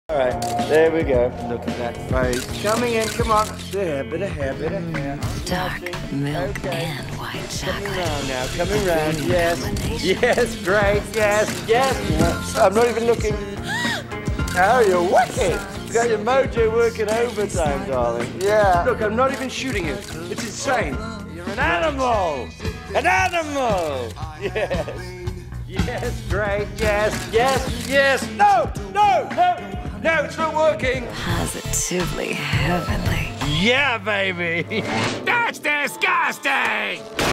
The Green M&M Is Too Sexy!
The Green M&M is getting a makeover. We couldn't resist spoofing this change. What are your thoughts, prefer the old look, or is candy just candy regardless?

Peacemaker Is Hilarious!